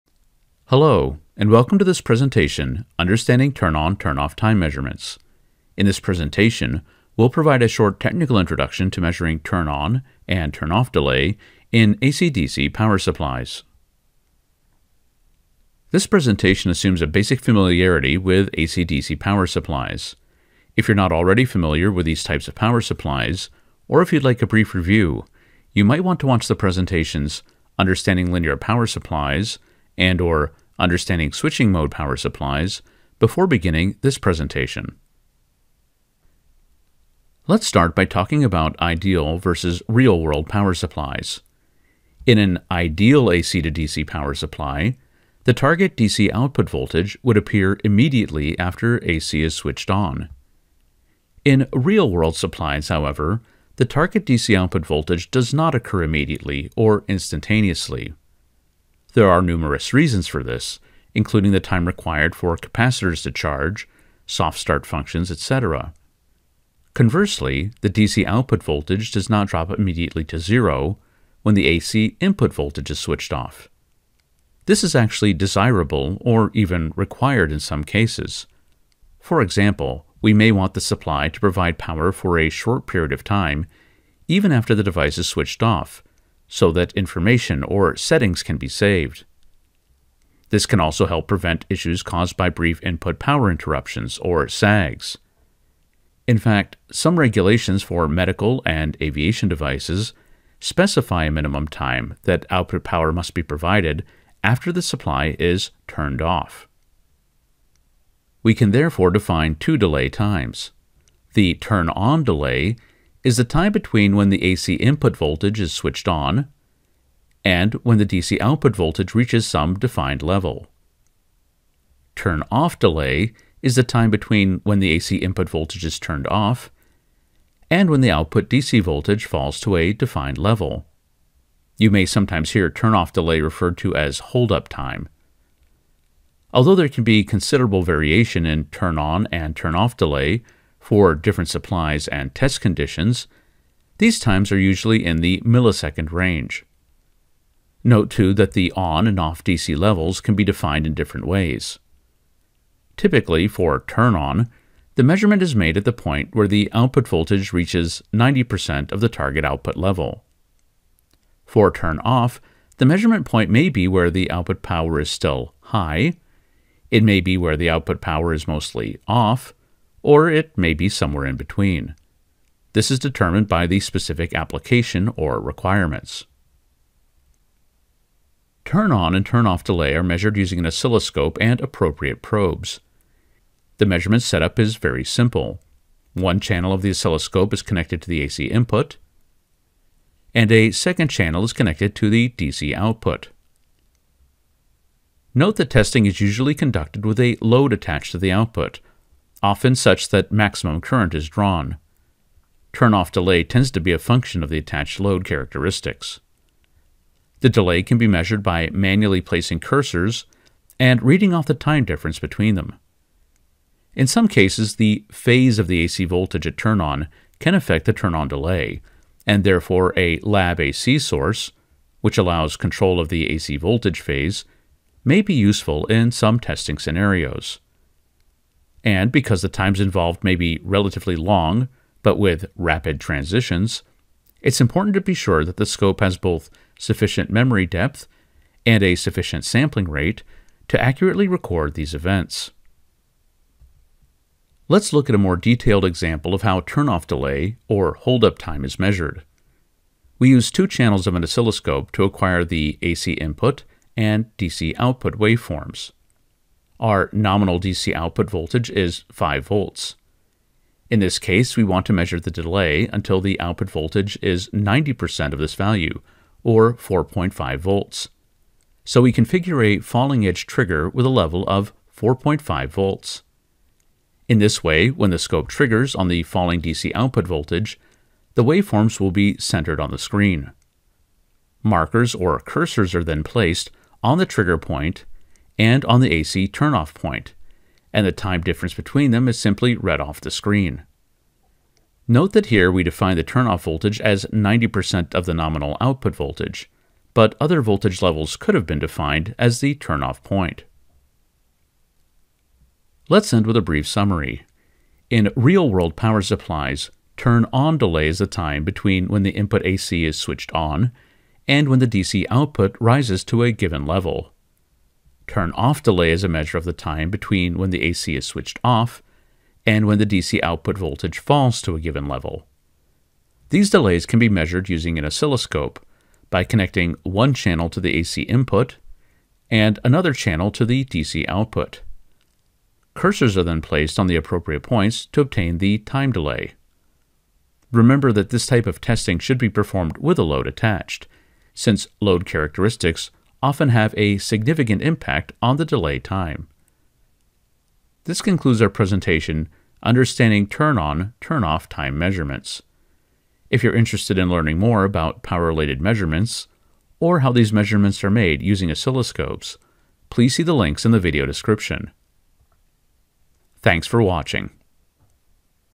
Understanding Turn On Turn Off Time Measurements
This video provides a short technical introduction to measuring turn-on and turn-off delay in AC DC power supplies.

Timeline:
00:00 Introduction
00:14 Suggested viewing
00:36 Ideal vs. real-world AC-DC power supplies
01:47 Defining “turn on / turn off” (delay) time
02:57 Measuring turn on / turn off delay
04:03 Measurement example (turn off delay)
05:05 Summary